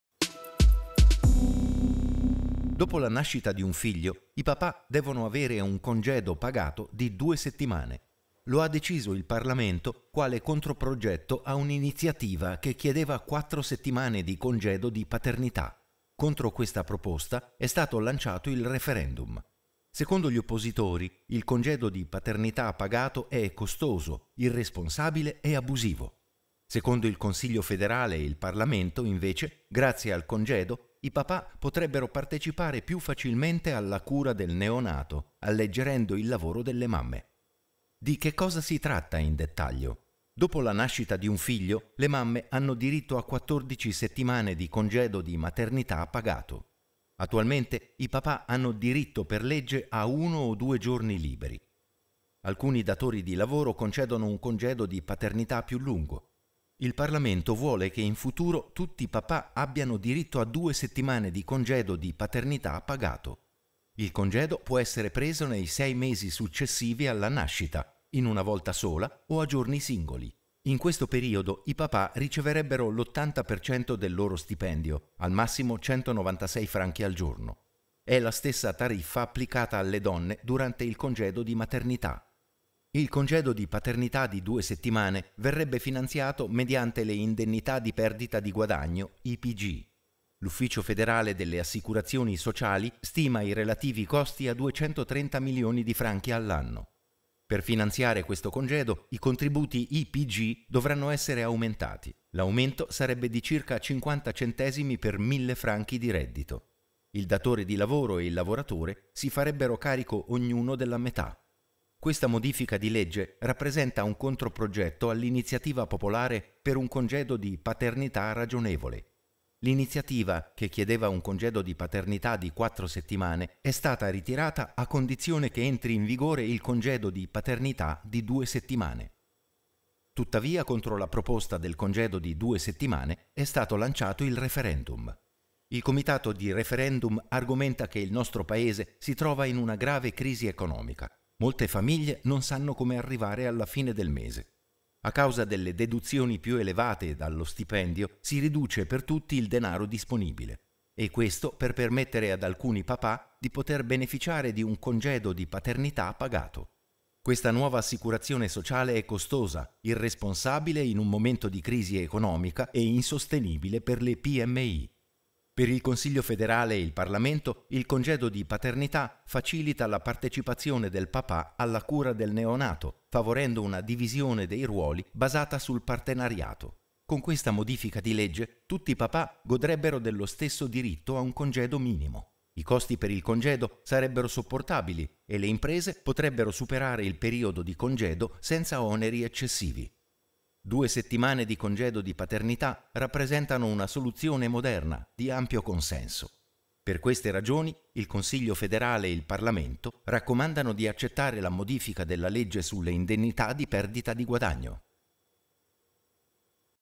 Lingua dei segni: Congedo di paternità pagato
Modifica della legge sulle indennità di perdita di guadagno (congedo di paternità pagato); Votazione popolare del 27 settembre 2020